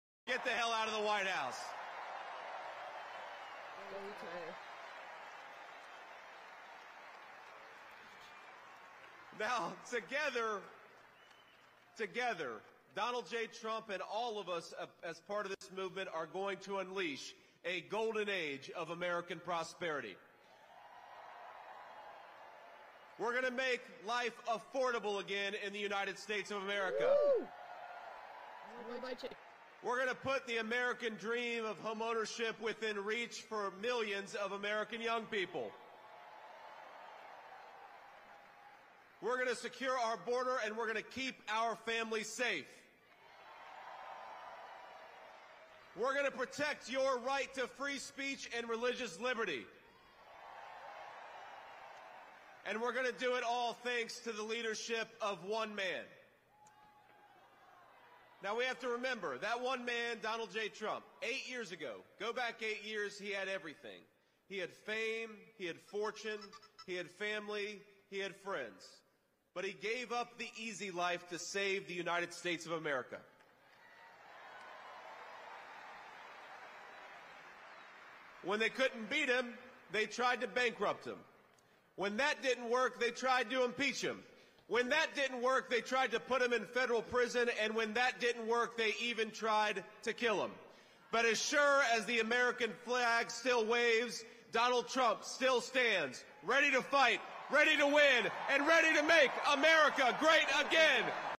Trump's Unyielding Spirit A Tribute by J D Vance
Trump Triumphs at MSG: A Supporter's View
The energy was electric as Trump supporters filled Madison Square Garden to capacity. The iconic venue, typically known for concerts and sports events, transformed into a sea of red MAGA hats and patriotic attire.
Opening Acts
The rally kicked off with a series of dynamic speakers who warmed up the crowd. Each speaker delivered powerful messages that resonated with the audience, touching on key issues important to Trump supporters. The crowd responded with enthusiasm, cheering and applauding throughout the opening acts.
Main Event
When former President Trump took the stage, the arena erupted in thunderous applause. His entrance was nothing short of spectacular, befitting the grand setting of Madison Square Garden. Trump's speech was filled with his signature charisma and straight talk, addressing topics such as:
Economic policies and job creation
Border security and immigration reform
America First foreign policy
Energy independence
The audience hung on every word, frequently breaking into chants. Trump's ability to connect with his supporters was on full display, as he effortlessly moved between serious policy discussions and lighthearted moments.
Unexpected Ending
As the rally reached its crescendo, with Trump delivering some of his most powerful points, the unexpected happened. This abrupt ending left many online viewers, including myself, frustrated and searching for alternative ways to continue watching the event.
Despite the technical difficulties with the stream, the rally itself was a resounding success, showcasing Trump's enduring popularity and the enthusiasm of his support base, even in the heart of New York City.